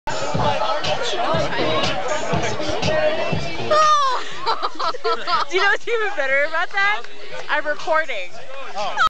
So Not Sexy
Relay For Life 2008 Mikey thought he would try and be a little sexy while wearing a wig. Didn't work too well.